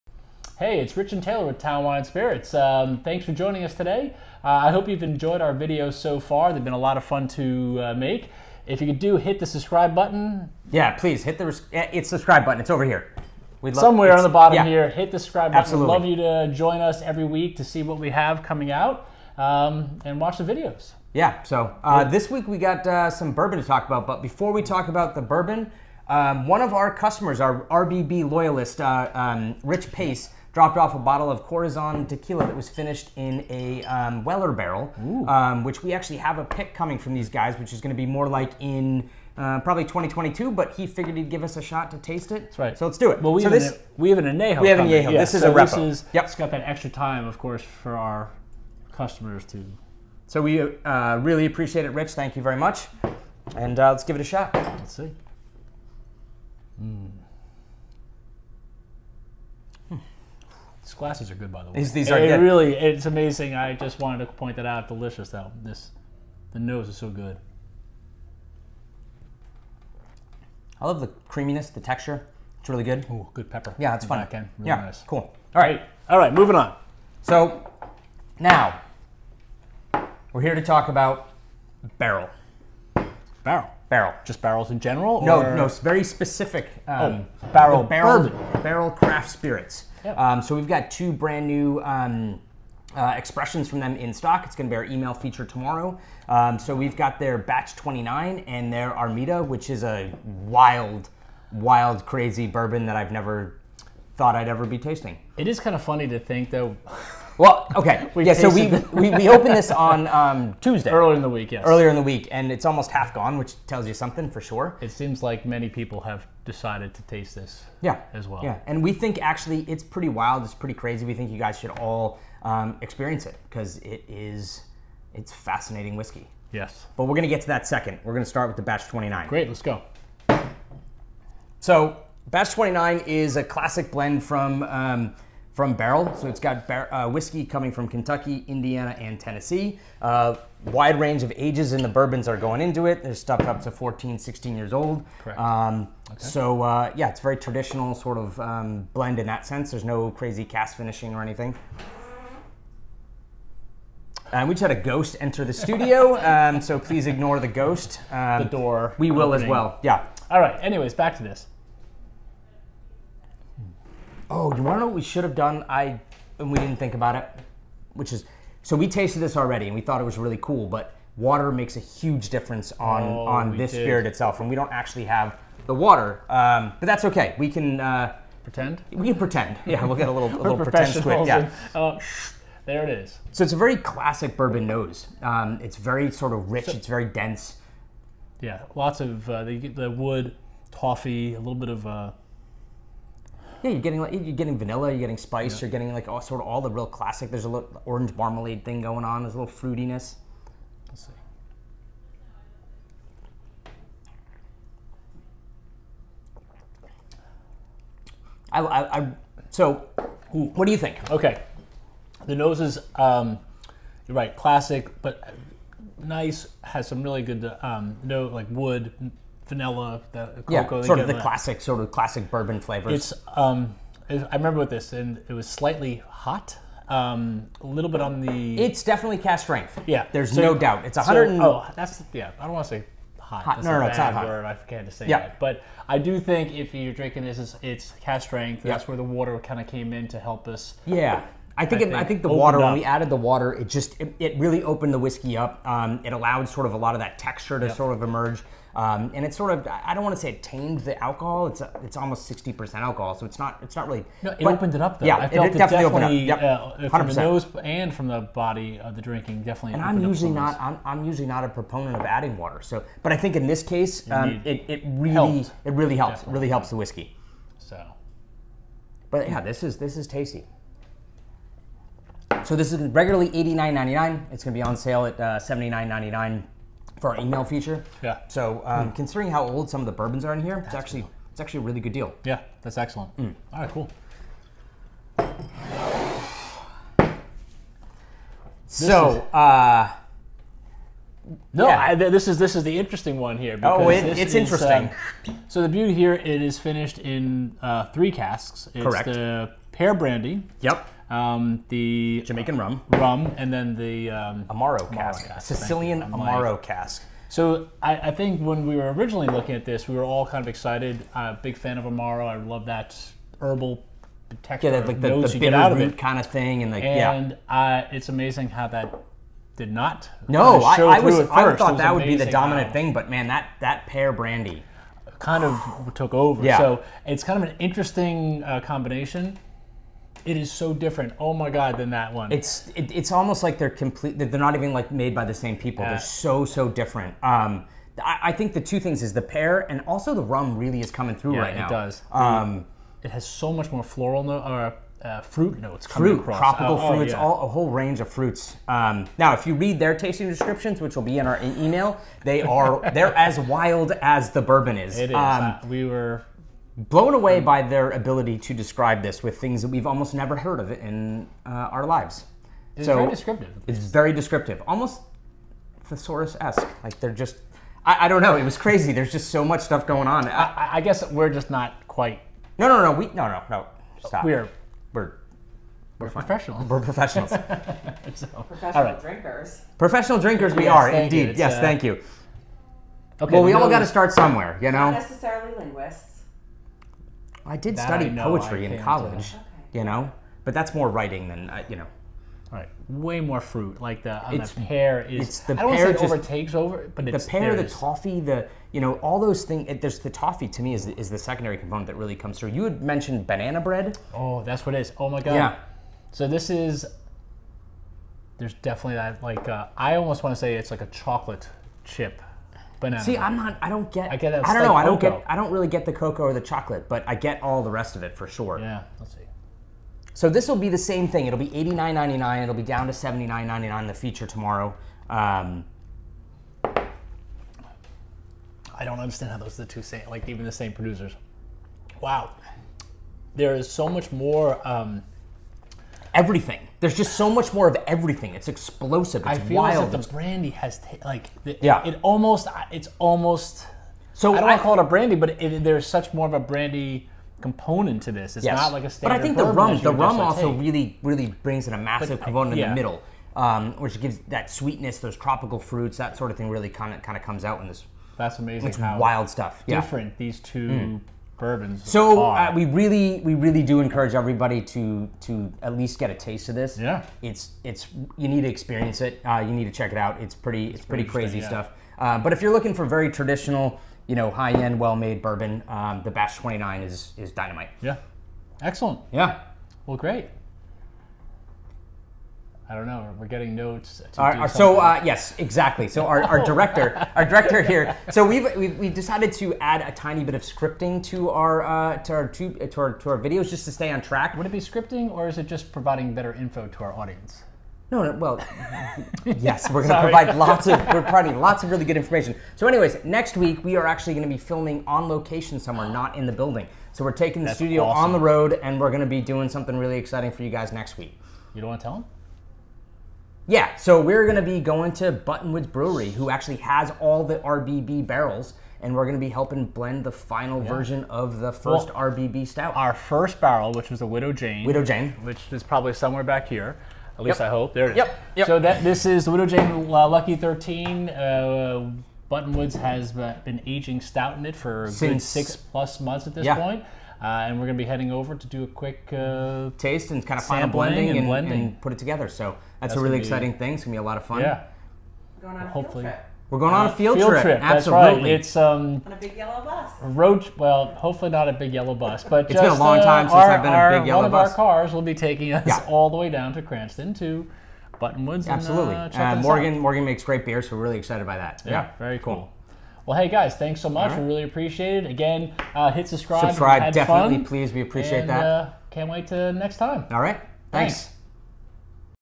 Tasting Bourbon! Barrell Batch 29 Bourbon and Armida Cask Finished Bourbon!
Rich and Taylor taste two new expressions from the Barrell Craft Spirits. The whiskies are so different from each other which really shows the massive impact cask finishing can have on a whiskey.